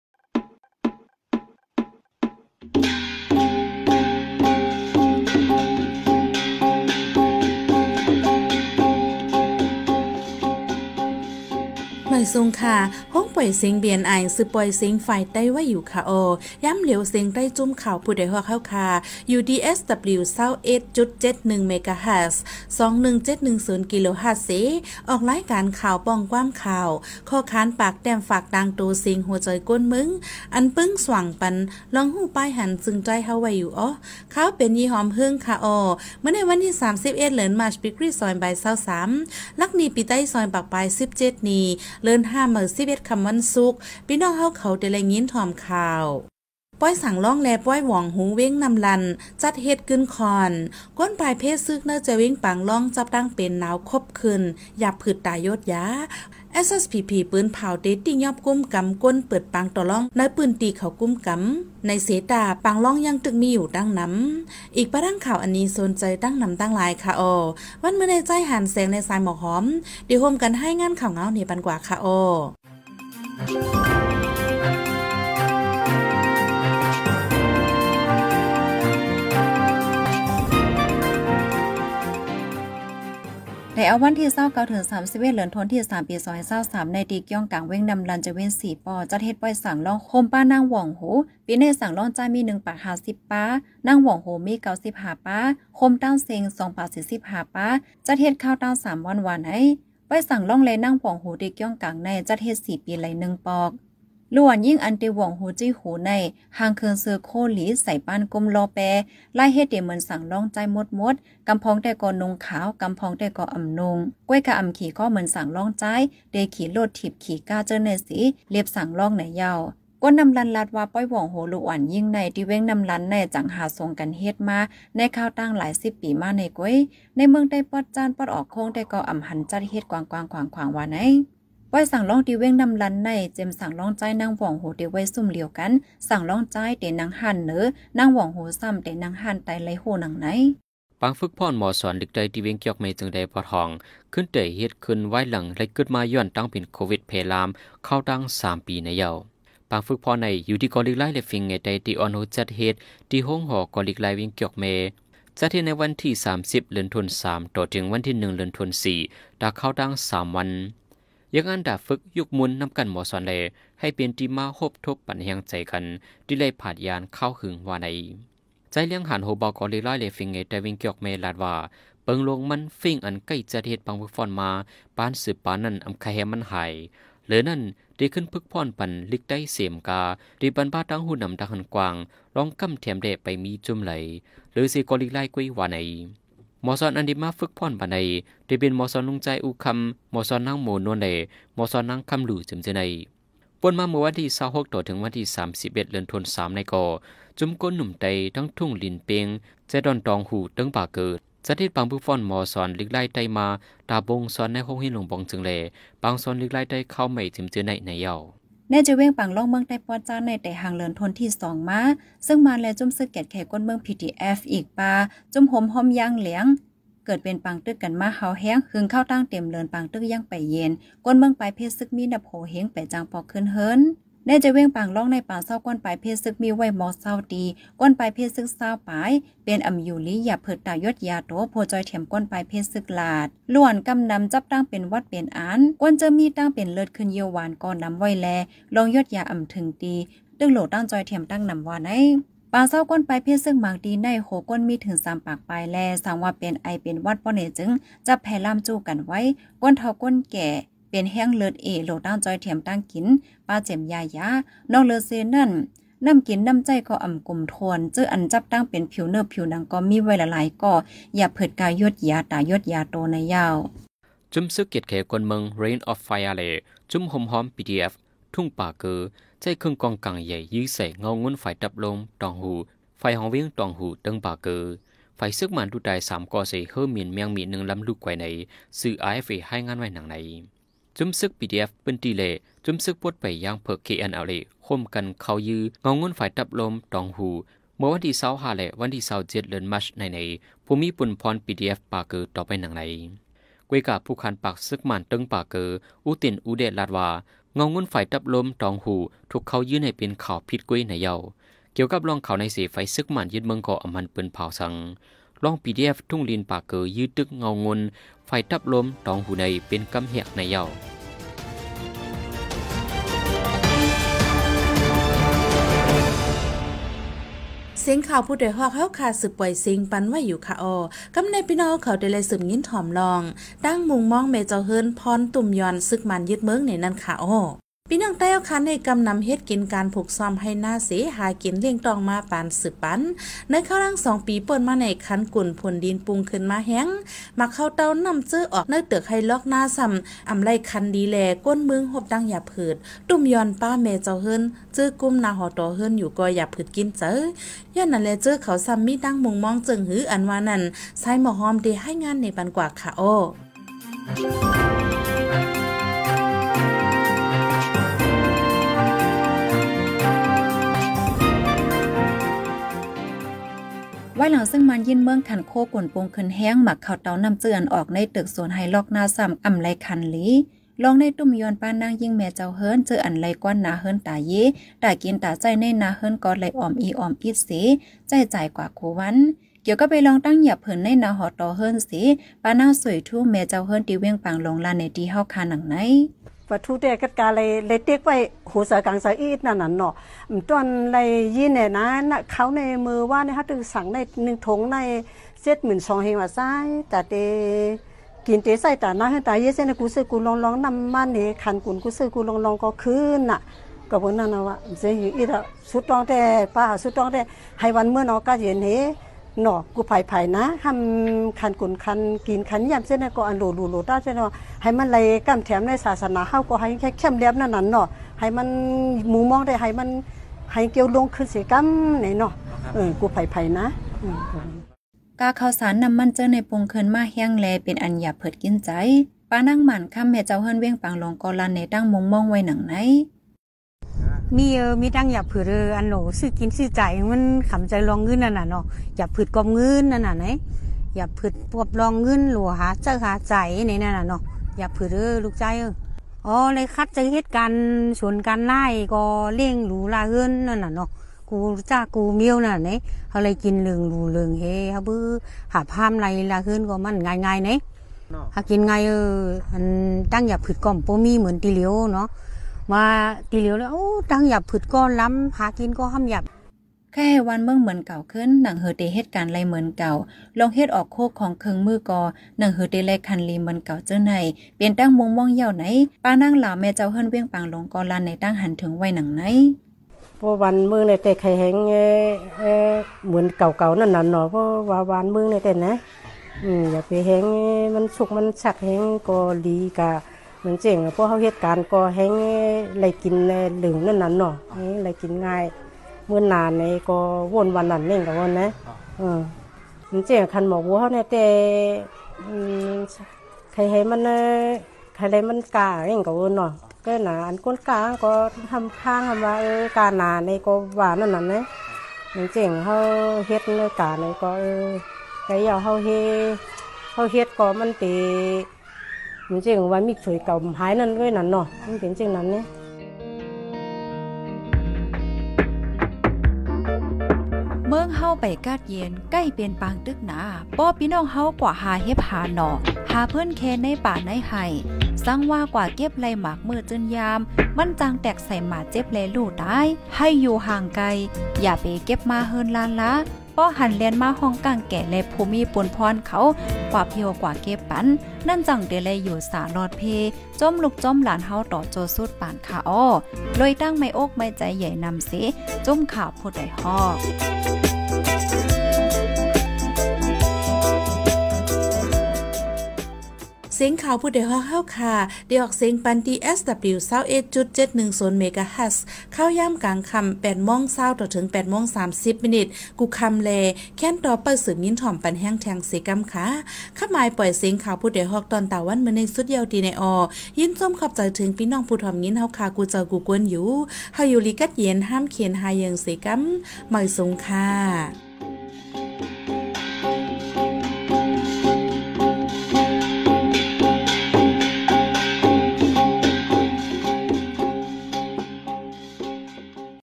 ပွႆးသၢင်ႇလွင်းလႄႈပွႆးဝွင်ႇႁူ ၸီးႁူ တီႈဝဵင်းၼမ်ႉလၼ်ႈပီၼႆႉ ၸတ်းႁဵတ်းၵိုၼ်းၶွၼ်ႈ
- ၵူၼ်းပၢႆႈၽေးသိုၵ်းၼႂ်းၸႄႈဝဵင်းပၢင်လွင်း ၸပ်းတၢင်းပဵၼ်ၼၢဝ်ၶူပ်းၶိုၼ်ႈ ယၢပ်ႇၽိုတ်ႇတႃႈယူတ်းယႃ
- SSPP ပိုၼ်ၽၢဝ်ႇတေတီႉၺွပ်းပၢင်တေႃႇလွင်းၼႂ်းၼႃႈတီႈၶဝ်ၵုမ်းၵမ် ဝႃႈၼႆသေတႃႉ တေႃႇထိုင်ယၢမ်းလဵဝ်ပၢင်တေႃႇလွင်းတိုၵ်ႉလိူင်ႇၼမ်
ဢိၵ်ႇပႃးတင်းၶၢဝ်ႇဢၼ်လီသူၼ်ၸႂ်တင်းၼမ်ၶႃႈဢေႃႈ။